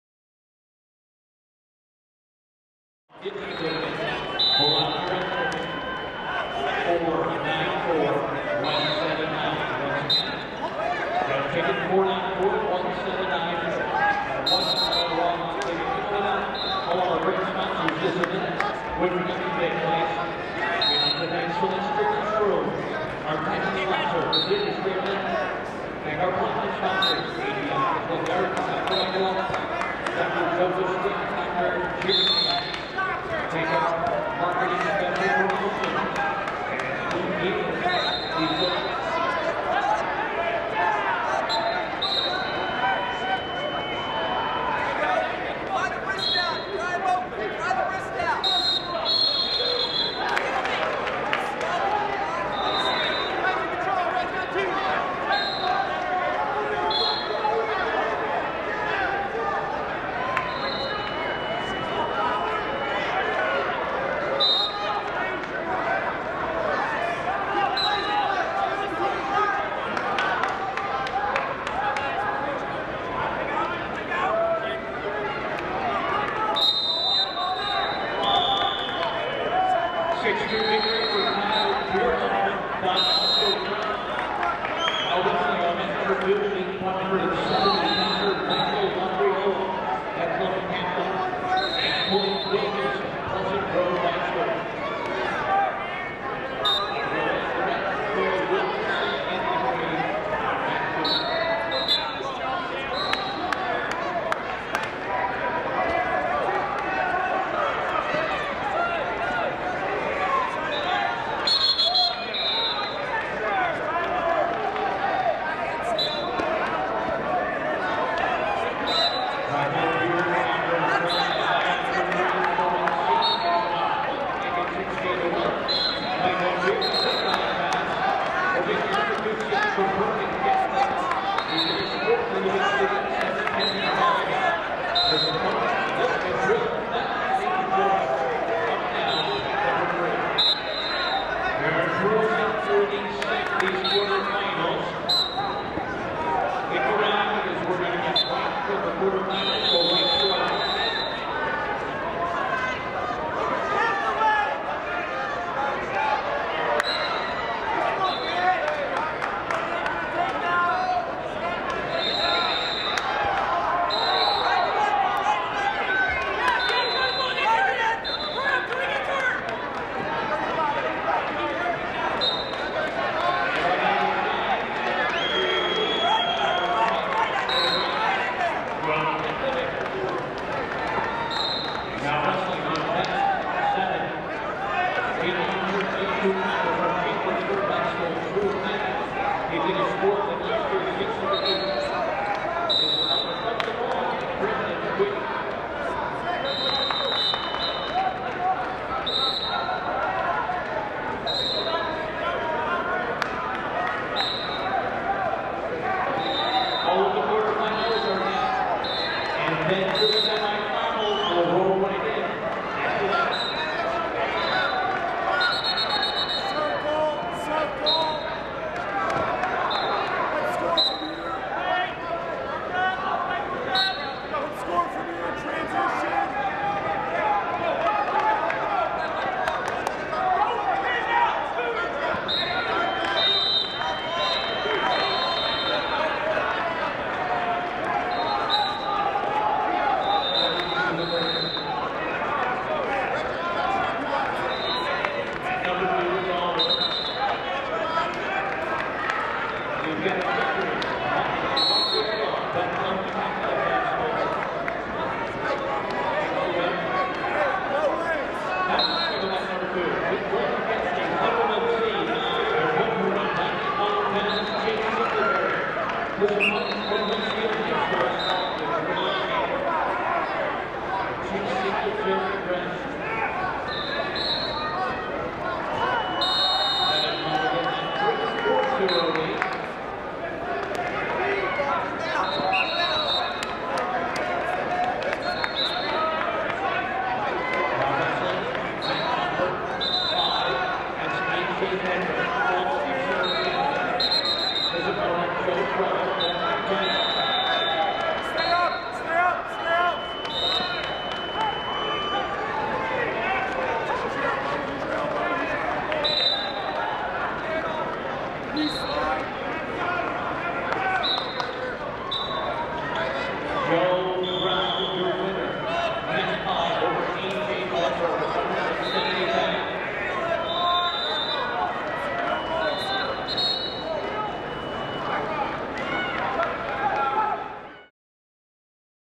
Beast of the East: Spencer Carey defeats Anthony Olivieri, 170lbs
Spencer Carey of DePaul defeated Anthony Olivieri of Hanover Park in quarterfinal action by a score of 7-2. The annual Beast of the East tournament is held at the University of Delaware and draws teams from across the country. (Video by Andre Malok |